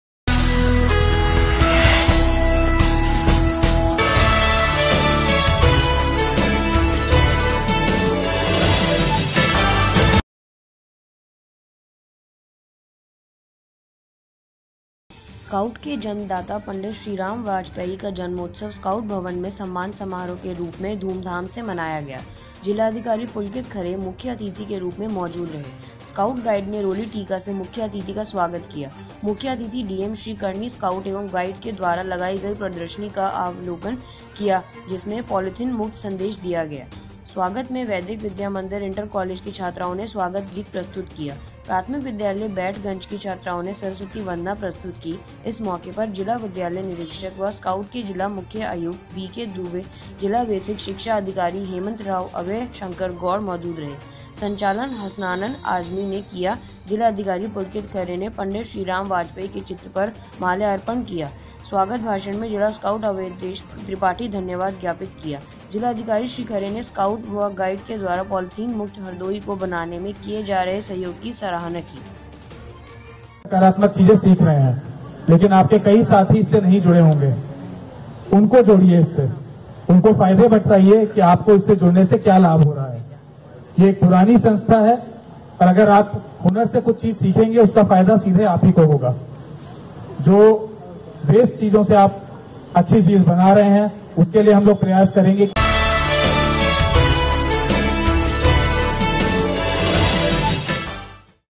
स्काउट के जन्मदाता पंडित श्रीराम वाजपेई का धूमधाम से मनाया गया जन्मोत्सव-Today24
स्काउट के जन्मदाता पंडित श्रीराम वाजपेई का जन्मोत्सव स्काउट भवन में सम्मान समारोह के रूप में धूमधाम से मनाया गया जिलाधिकारी पुलकित खरे मुख्य अतिथि के रुप में मौजूद रहे स्काउट गाइड ने रोली टीका से मुख्य अतिथि का स्वागत किया मुख्य अतिथि DM श्री करणी स्काउट एवं गाइड के द्वारा लगाई गई प्रदर्शनी का अवलोकन किया जिसमें पॉलिथीन मुक्त संदेश दिया गया स्वागत में वैदिक विद्या मंदिर इंटर कॉलेज की छात्राओं ने स्वागत गीत प्रस्तुत किया प्राथमिक विद्यालय बैठ गंज की छात्राओं ने सरस्वती वंदना प्रस्तुत की इस मौके पर जिला विद्यालय निरीक्षक व स्काउट के जिला मुख्य आयुक्त बीके दुबे जिला बेसिक शिक्षा अधिकारी हेमंत राव अभय शंकर गौड़ मौजूद रहे  संचालन हसनान आजमी ने किया जिलाधिकारी पुलकित खरे ने पंडित श्रीराम वाजपेई के चित्र पर माल्यार्पण किया स्वागत भाषण में जिला स्काउट  अवधेश त्रिपाठी धन्यवाद ज्ञापित किया। जिलाधिकारी श्री खरे ने  स्काउट व गाइड  के द्वारा पालीथीन मुक्त हरदोई को बनाने में किए जा रहे सहयोग की सराहना की।

T.V. News

Application.

views!!!

News."